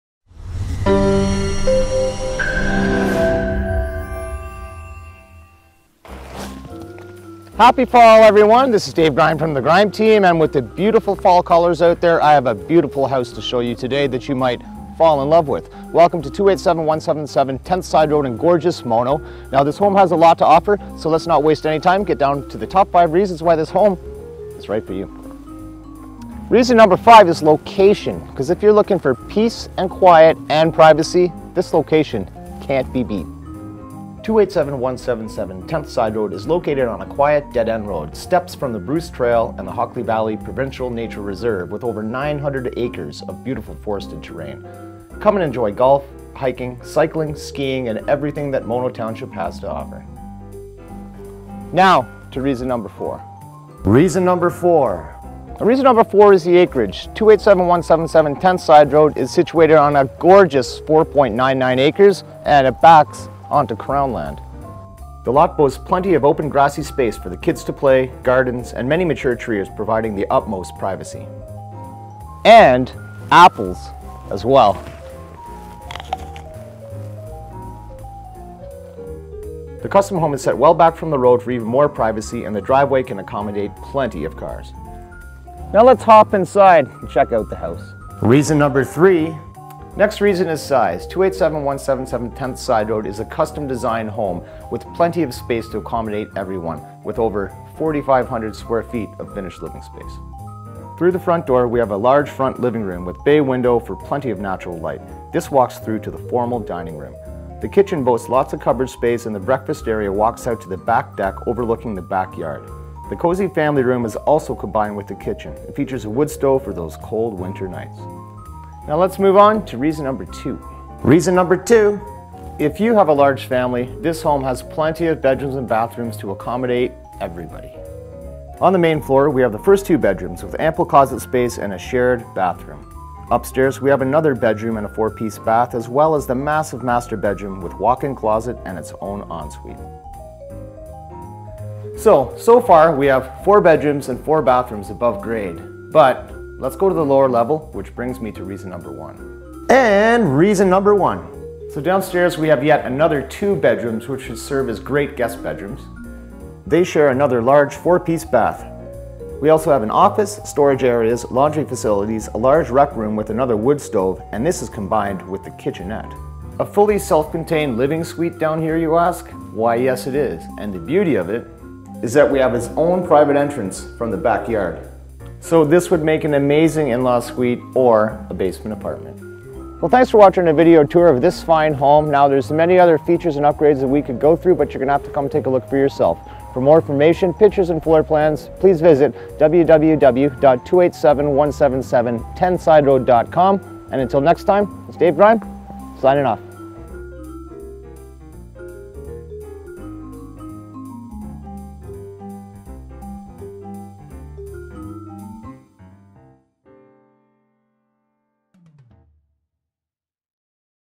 New Real Estate Video: 287177 10 sideroad Mono, ON, The Grime Team
In The Rolling Hills Of Mono This 4.99 Acre Property Affords Privacy And Nature At Its Best On A Cul De Sac With Close Proximity (280m) To Hockley Valley Provincial Nature Reserve (Over 900 Acres) And Bruce Trail - Minutes To Thriving Orangeville. This 2724 Sq.Ft. Custom Designed 2-Storey Home (1980) Has an Additional 1600 Sq. Ft. Finish Down (2 Bedroom + Bath, Living Area, Office + Kitchenette with Separate Entrance – Ideal for A Multi-Generational Family. EXTRAS Open Concept Kit/Fr W/Cozy Woodstove & W/O To Extensive Decking. Sep Liv/Din & 2 Br On Main Floor. Upper Level Mbr W/Dormer Windows, Wicc & Ensuite. Plus 4th Br & Bath. Shingles’17. Apple Trees.